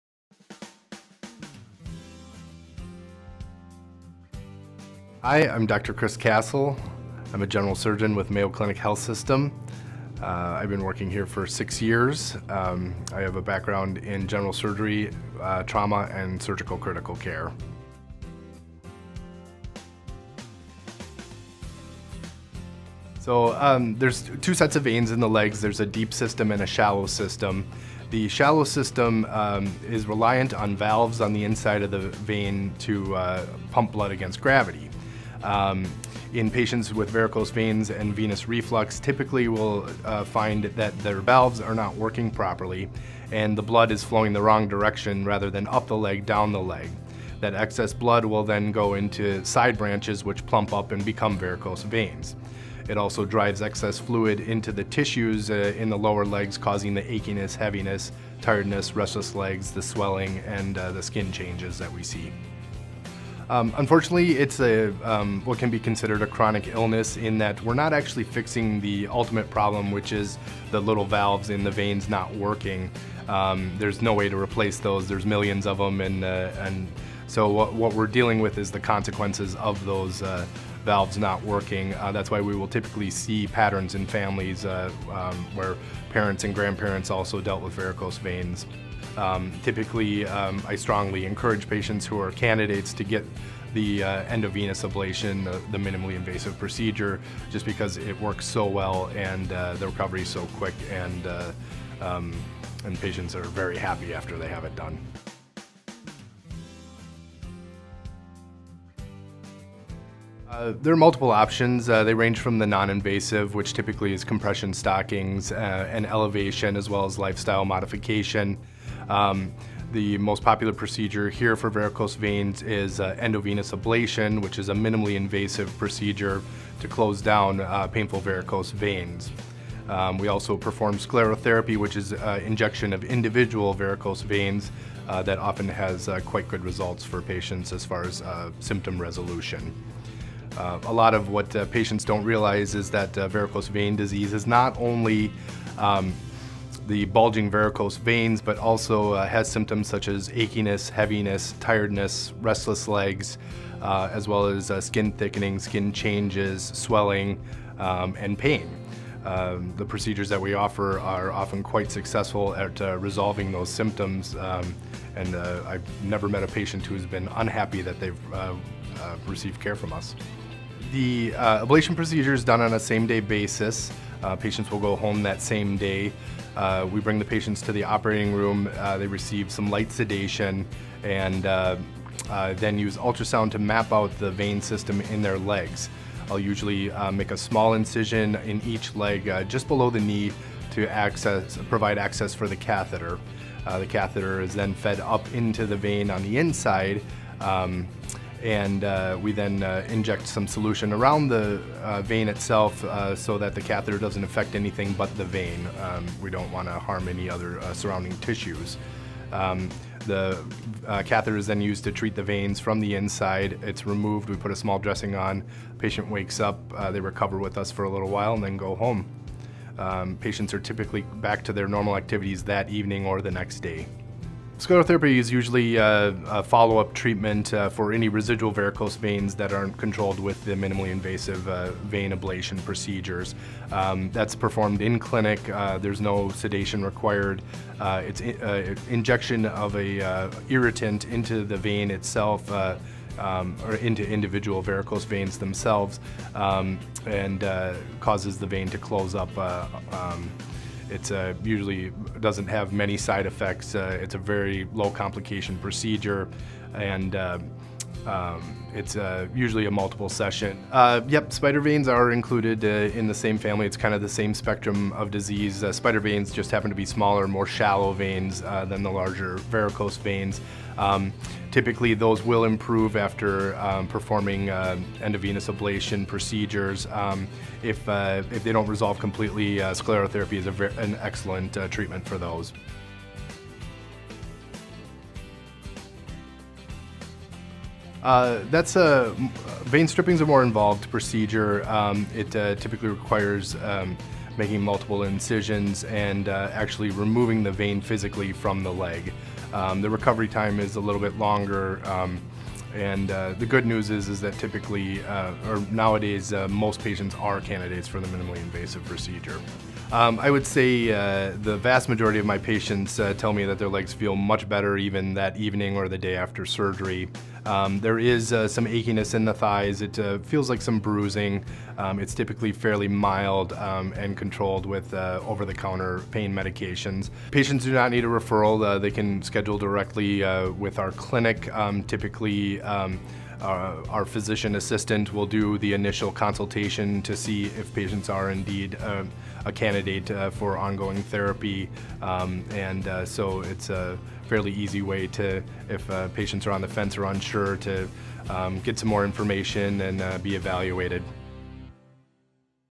Christopher Kasal, M.D.
Christopher Kasal, M.D., a general surgeon at Mayo Clinic Health System, discusses common concerns about varicose veins and treatment options.